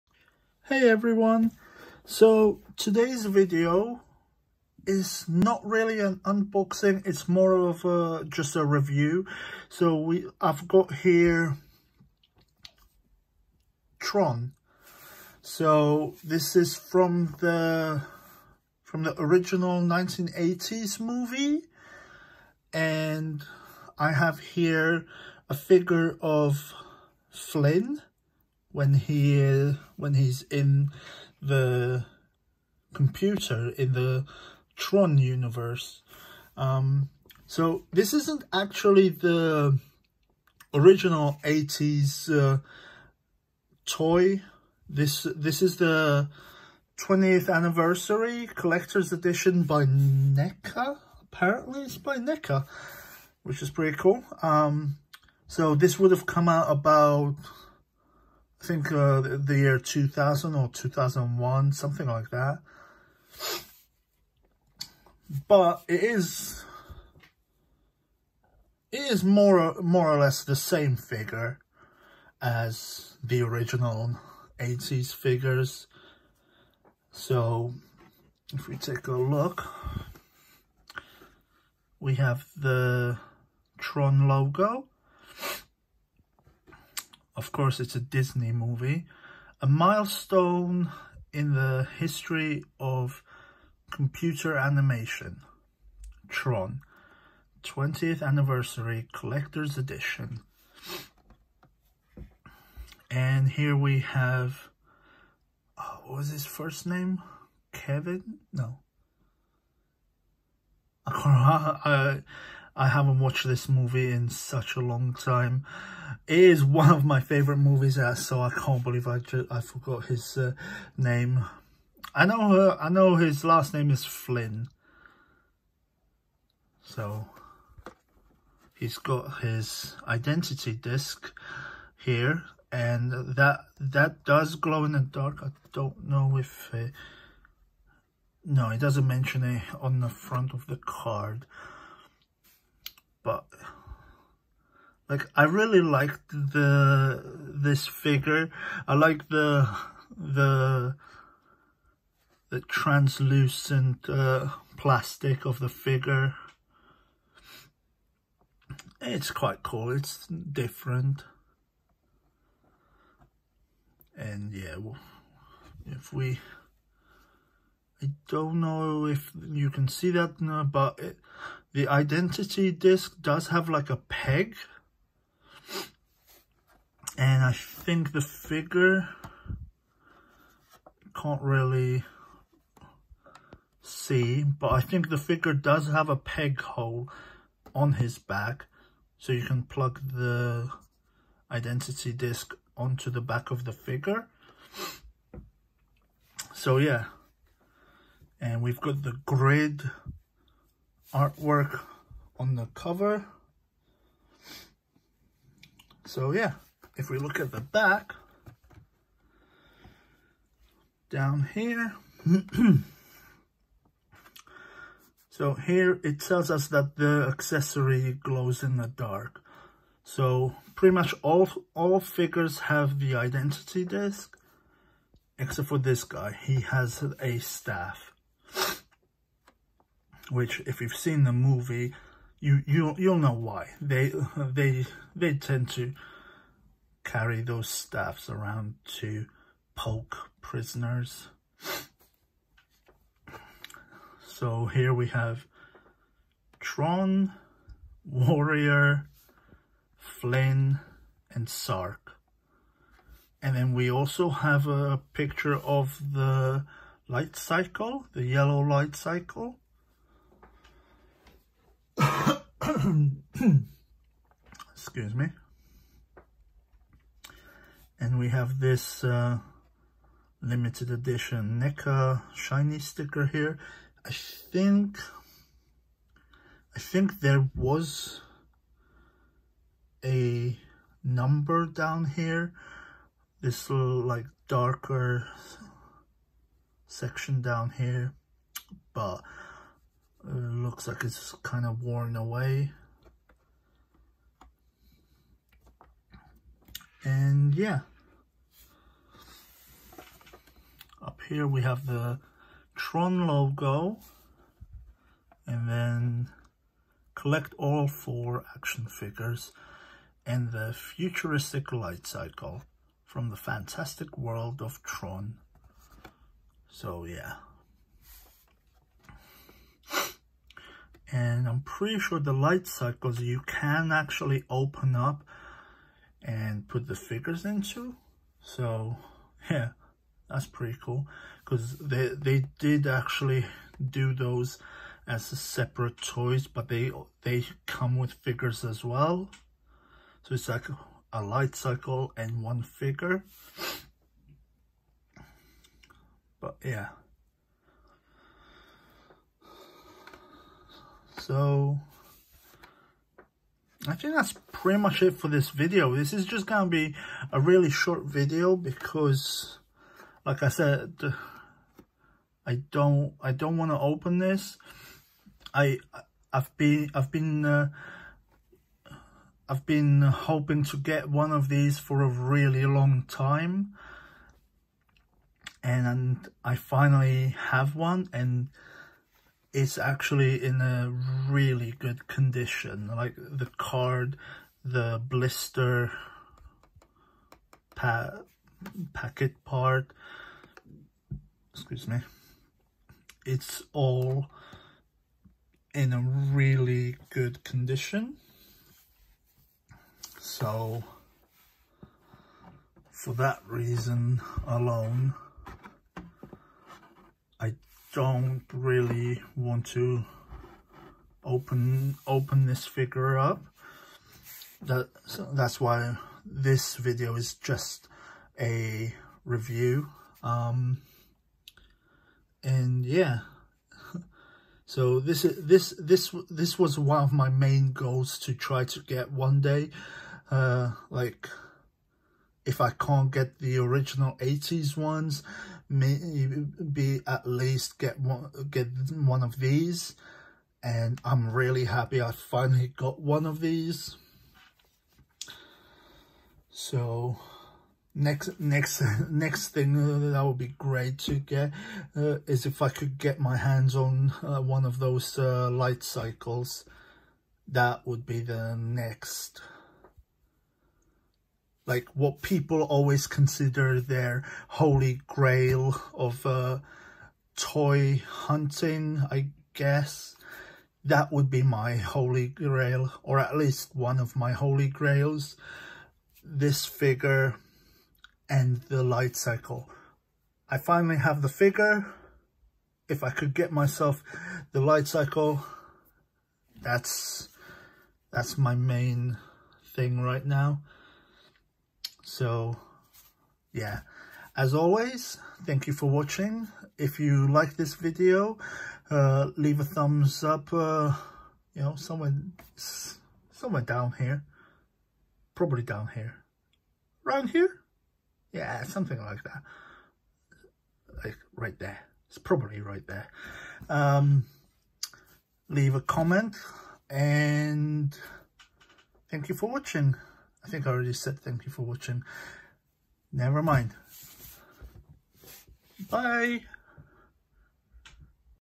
TRON 1982 ( 20th anniversary collector’s edition ) Kevin Flynn figure review. 😊
It was Kevin I did get the name right the first time lol 😆

Really happy I have one of these. 😊